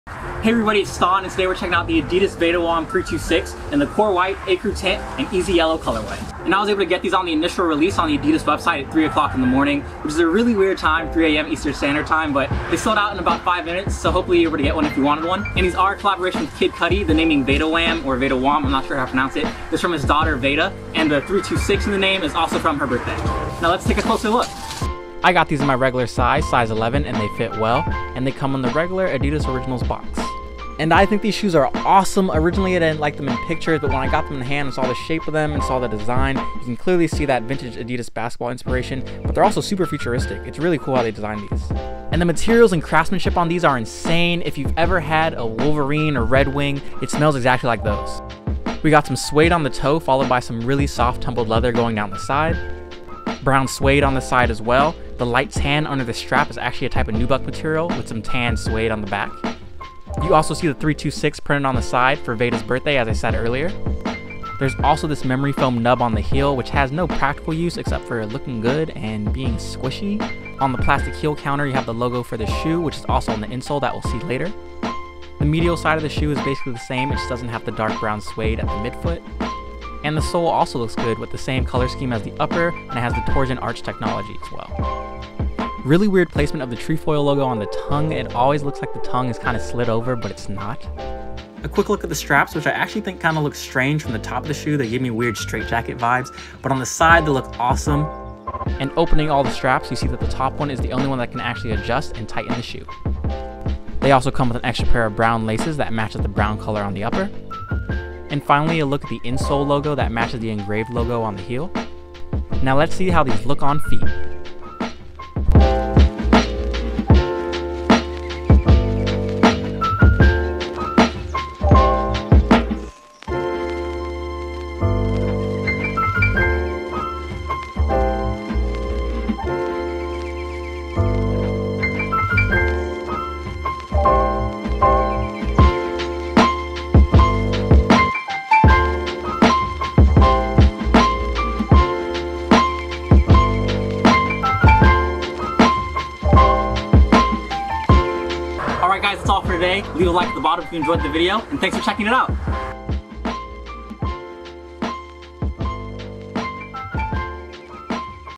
Kid Cudi x Adidas Vadawam 326 - 'Core White / Ecru Tint / Easy Yellow' Detailed + On Foot Look!
Hey everybody! My name is Ston and this is my detailed, on foot look of the Kid Cudi x Adidas Vadawam 326 in the Core White / Ecru Tint / Easy Yellow colorway!